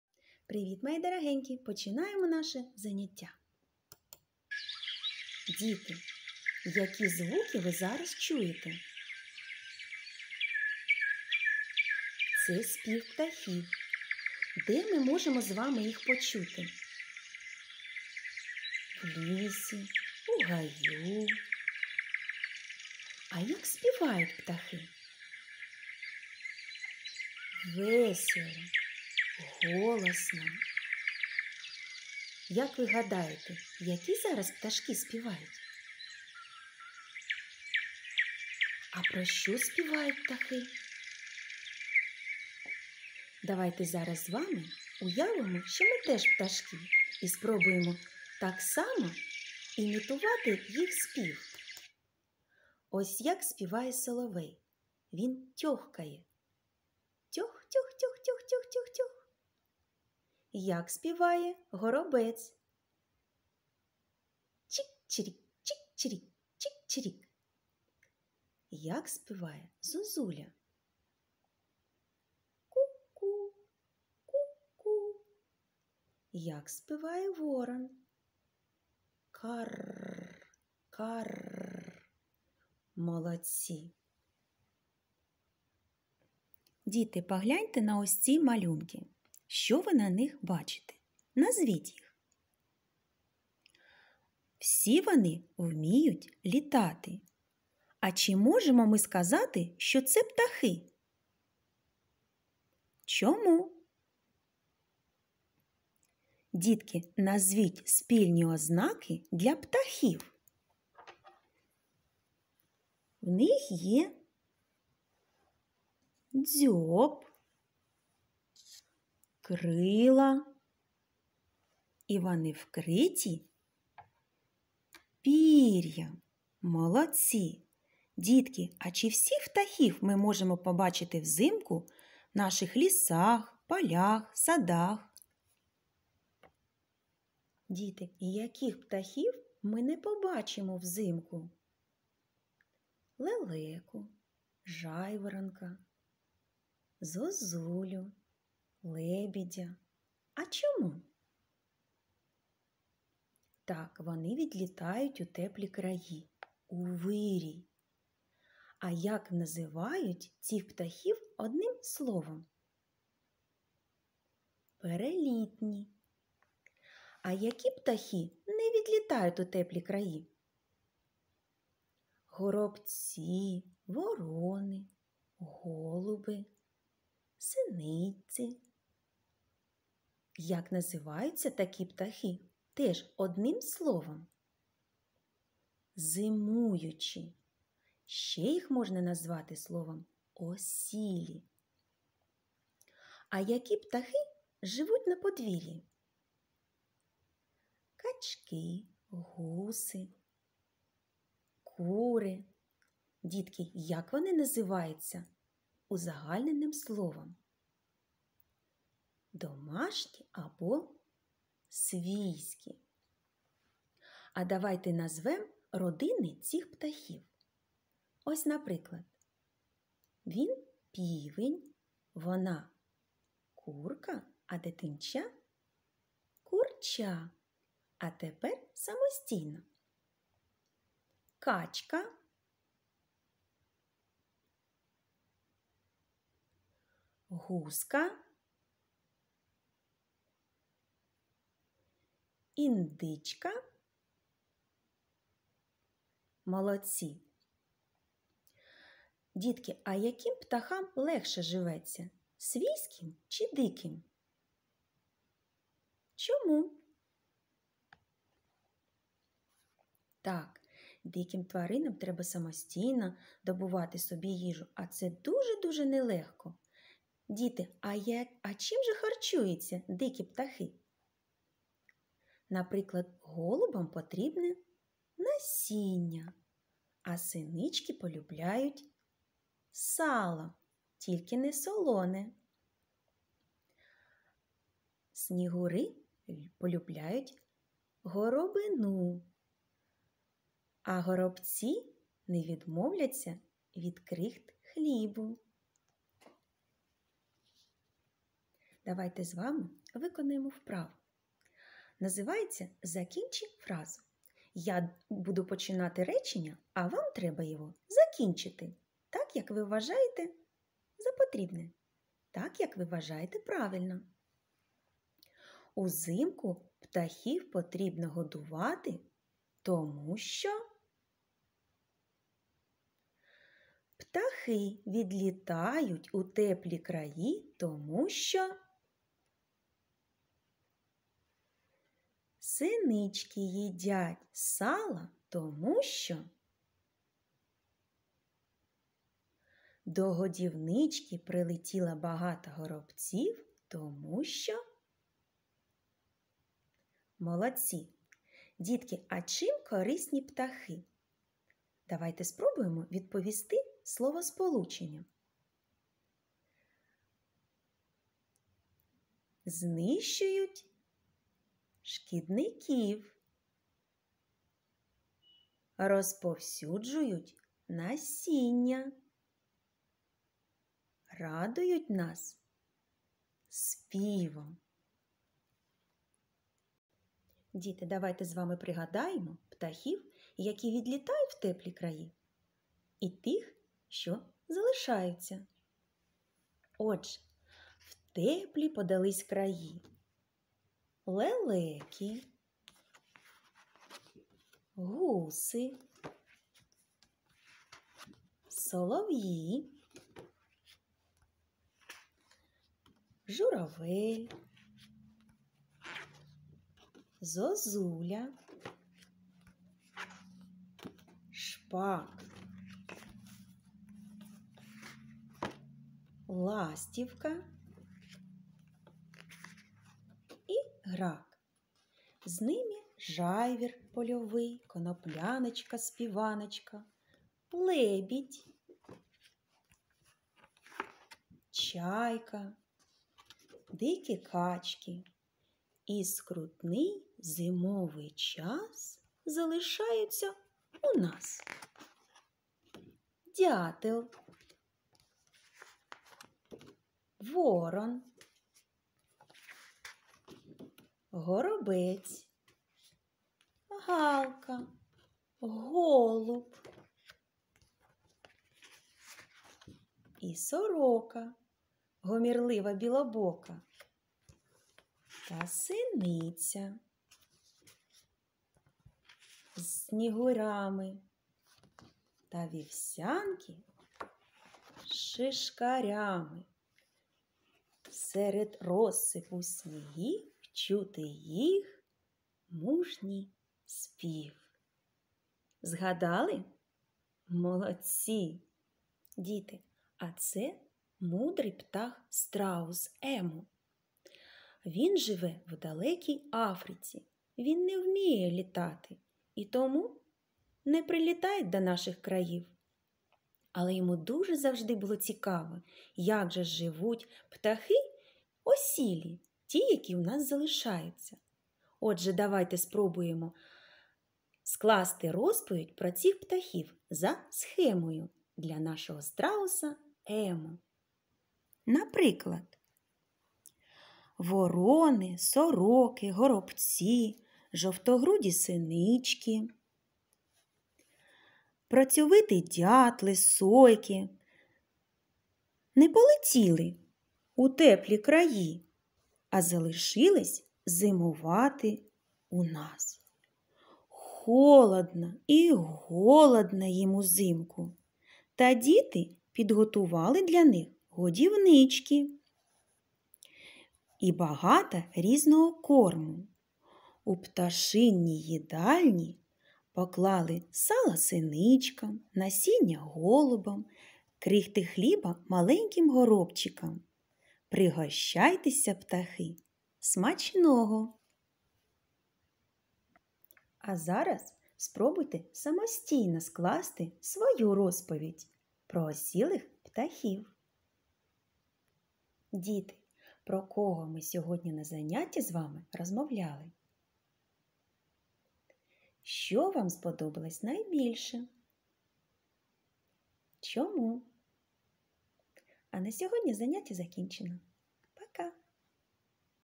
Розвиток мовлення. Птахи.
Збагачення лексики словами-назвами птахів, прикметами, дієсловами ознак, властивостей, узагальнювальні слова, слова родової належності; розвиток вміння складати розповідь за схемою (сюжетні картинки), виховання пізнавального інтересу до життя птахів та турботливе ставлення до них.